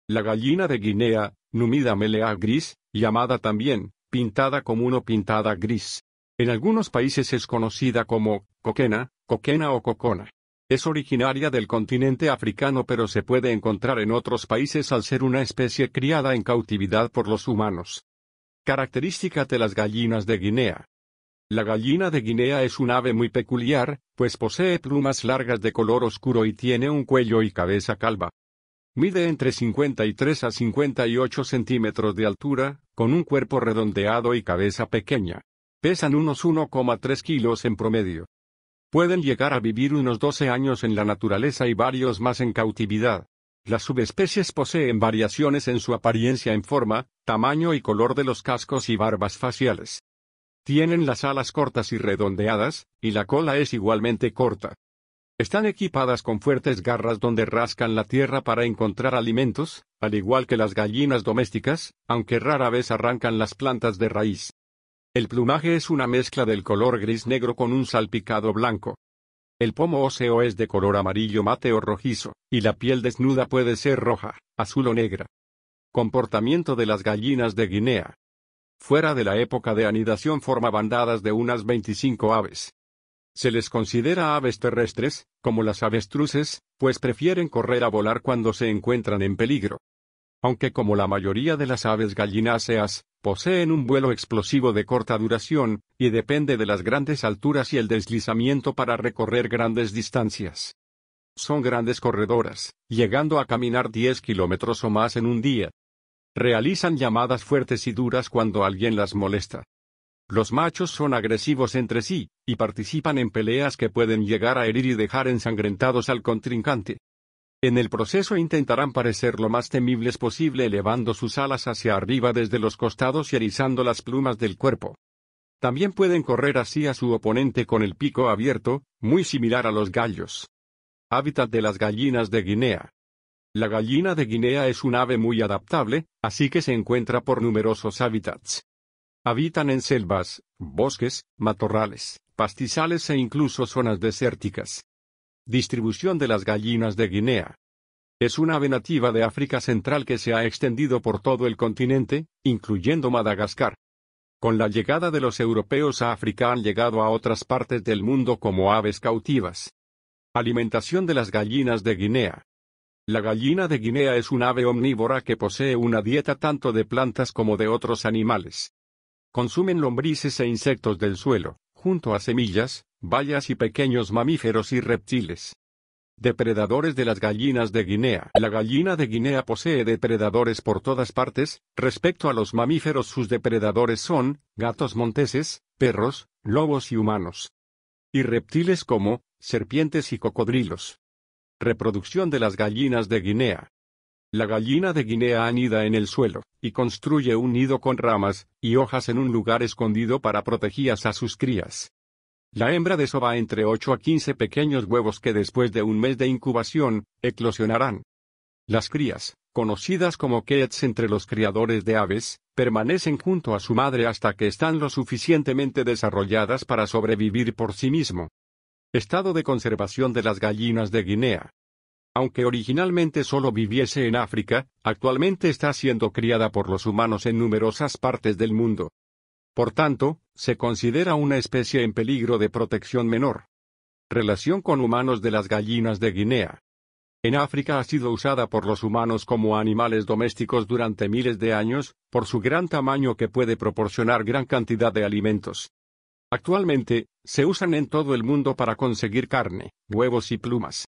GALLINAS DE GUINEA, Documental de las Gallinas de Guinea, Gallinas de Guinea Curiosidades
La gallina de Guinea (Numida meleagris) llamada también: pintada común o pintada gris.
En algunos países es conocida como: coquena,  kokena o cocóna.
Es originaria del continente africano pero se puede encontrar en otros países al ser una especie criada en cautividad por los humanos.

Características de las gallinas de Guinea.

La gallina de Guinea es un ave muy peculiar,  pues posee plumas largas de color oscuro y tiene un cuello y cabeza calva.
Mide entre 53 a 58 cm de altura,  con un cuerpo redondeado y cabeza pequeña.  Pesan unos 1,3 kg en promedio.
Pueden llegar a vivir unos 12 años en la naturaleza y varios más en cautividad.  Las subespecies poseen variaciones en su apariencia en forma,  tamaño y color de los cascos y barbas faciales.

Tienen las alas cortas y redondeadas,  y la cola es igualmente corta.
Están equipadas con fuertes garras donde rascan la tierra para encontrar alimentos,  al igual que las gallinas domésticas,  aunque rara vez arrancan las plantas de raíz.

El plumaje es una mezcla del color gris-negro con un salpicado blanco.
El pomo óseo es de color amarillo mate o rojizo,  y la piel desnuda puede ser roja,  azul o negra.

Comportamiento de las gallinas de Guinea.

Fuera de la época de anidación forma bandadas de unas 25 aves.
Se les considera aves terrestres,  como las avestruces,  pues prefieren correr a volar cuando se encuentran en peligro.
Aunque como la mayoría de las aves gallináceas,  poseen un vuelo explosivo de corta duración y depende de las grandes alturas y el deslizamiento para recorrer grandes distancias.

Son grandes corredoras,  llegando a caminar 10 km o más en un día.
Realizan llamadas fuertes y duras cuando alguien las molesta.

Los machos son agresivos entre sí y participan en peleas que pueden llegar a herir y dejar ensangrentados al contrincante.
En el proceso intentarán parecer lo más temibles posible elevando sus alas hacia arriba desde los costados y erizando las plumas del cuerpo.
También pueden correr hacía su oponente con el pico abierto,  muy similar a los gallos.

Hábitat de las gallinas de Guinea.

La gallina de Guinea es un ave muy adaptable,  así que se encuentra por numerosos hábitats.
Habitan en selvas,  bosques,  matorrales,  pastizales e incluso zonas desérticas.

Distribución de las gallinas de Guinea.

Es un ave nativa de África Central que se ha extendido por todo el continente,  incluyendo Madagascar.
Con la llegada de los europeos a África han llegado a otras partes del mundo como aves cautivas.

Alimentación de las gallinas de Guinea.

La gallina de Guinea es un ave omnívora que posee una dieta tanto de plantas como de otros animales.
Consumen lombrices e insectos del suelo,  junto a semillas,  bayas y pequeños mamíferos y reptiles.

Depredadores de las gallinas de Guinea.

La gallina de Guinea posee depredadores por todas partes,  respecto a los mamíferos sus depredadores son: gatos monteses,  perros,  lobos y humanos.
Y reptiles como: serpientes y cocodrilos.

Reproducción de las gallinas de Guinea.

La gallina de Guinea anida en el suelo y construye un nido con ramas y hojas en un lugar escondido para protegías a sus crías.
La hembra desova entre 8 a 15 pequeños huevos que después de un mes de incubación,  eclosionarán.
Las crías (conocidas como keets entre los criadores de aves) permanecen junto a su madre hasta que están lo suficientemente desarrolladas para sobrevivir por sí mismo.

Estado de conservación de las gallinas de Guinea.

Aunque originalmente sólo viviese en África,  actualmente está siendo criada por los humanos en numerosas partes del mundo.
Por tanto,  se considera una especie en Peligro de protección menor.

Relación con humanos de las gallinas de Guinea.

En África ha sido usada por los humanos como animales domésticos durante miles de años,  por su gran tamaño que puede proporcionar gran cantidad de alimentos.
Actualmente,  se usan en todo el mundo para conseguir carne,  huevos y plumas.